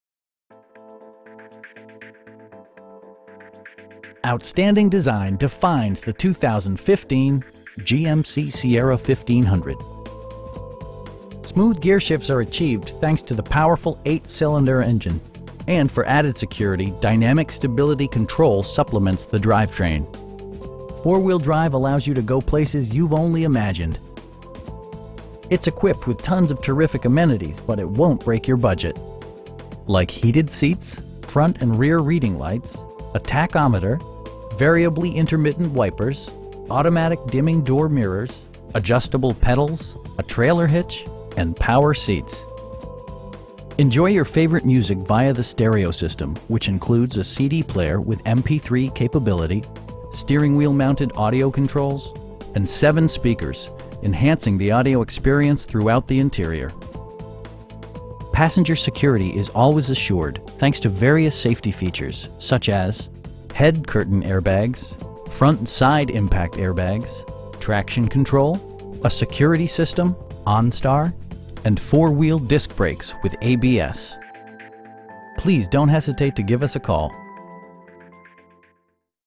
2015 GMC Sierra 1500 SLT in Jacksonville, FL 32256
Outstanding design defines the 2015 GMC Sierra 1500. Smooth gearshifts are achieved thanks to the powerful 8 cylinder engine, and for added security, dynamic Stability Control supplements the drivetrain. Four wheel drive allows you to go places you've only imagined. It's equipped with tons of terrific amenities, but it won't break your budget. Like heated seats, front and rear reading lights, a tachometer, variably intermittent wipers, automatic dimming door mirrors, adjustable pedals, a trailer hitch, and power seats. Passenger security is always assured thanks to various safety features, such as: head curtain airbags, front SIDE impact airbags, traction control, a security system, onStar, and 4 wheel disc brakes with ABS. We pride ourselves in the quality that we offer on all of our vehicles. Please don't hesitate to give us a call.